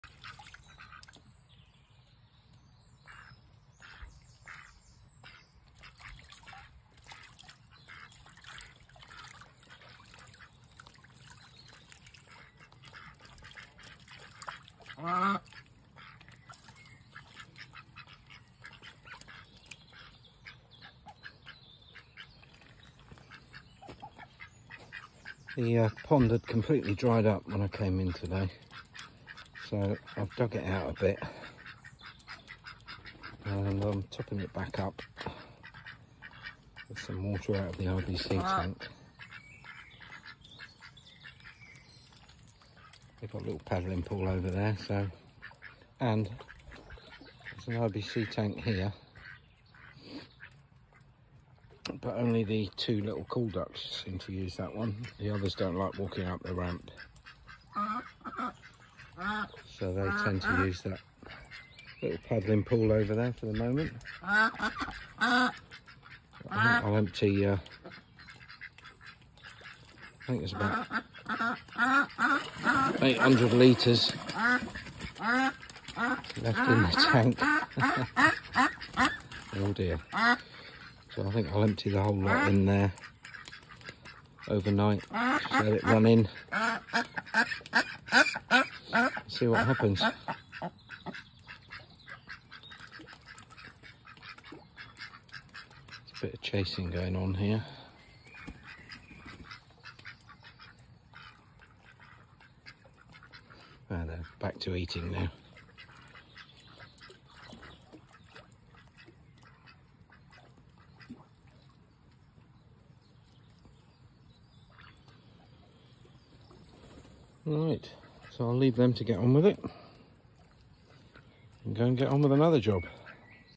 Splashing around in the duck run
The small duck pond had dried up today so I dug it out and started to refill it